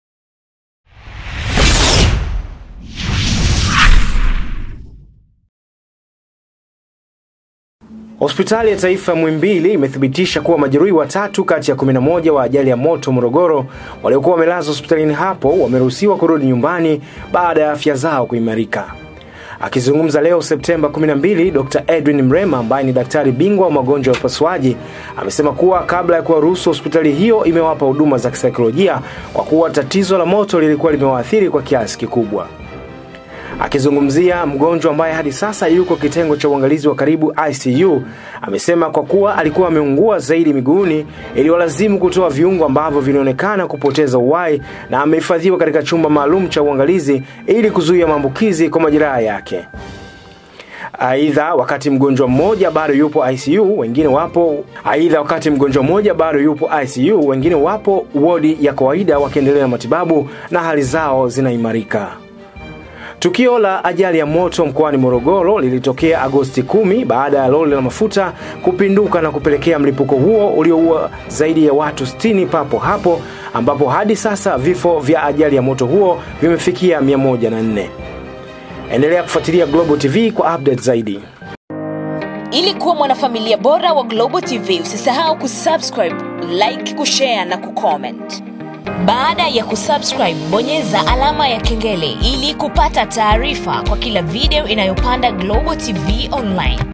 UPDATES: MAJERUHI AJALI YA MOTO WARUHUSIWA KURUDI NYUMBANI, MMOJA ABAKI ICU!
HOSPITALI ya Taifa Muhimbili (MNH) imethibitisha kuwa, majeruhi watatu kati ya 11 wa ajali ya moto Morogoro waliokuwa wamelazwa hospitalini hapo, wameruhusiwa kurudi nyumbani baada ya afya zao kuimarika.

Akizungumza leo Septemba 12, Dkt Edwin Mrema  ambaye ni Daktari Bingwa wa Magonjwa ya Upasuaji amesema kuwa, kabla ya kuwaruhusu  hospitali hiyo imewapa huduma za kisaikolojia kwa kuwa tatizo la moto lilikuwa limewathiri kwa kiasi kikubwa.

Akizungumzia mgonjwa ambaye hadi sasa yuko kitengo cha uangalizi wa karibu (ICU), amesema kwa kuwa alikuwa ameungua zaidi miguuni, iliwalazimu kutoa viungo ambavyo vilionekana kupoteza uhai na amehifadhiwa katika chumba maalumu cha uangalizi ili kuzuia maambukizi kwa majeraha yake.

Aidha wakati mgonjwa mmoja bado yupo ICU, wengine wapo wodi ya kawaida wakiendelea na matibabu na hali zao zinaimarika.

Tukio la ajali ya moto Mkoani Morogoro, lilitokea Agosti 10, baada ya lori la mafuta kupinduka na kupelekea mlipuko huo ulioua zaidi ya watu 60 papohapo, ambapo hadi sasa vifo vya ajali ya moto huo

Twitter Install Global App: ANDROID Install Global App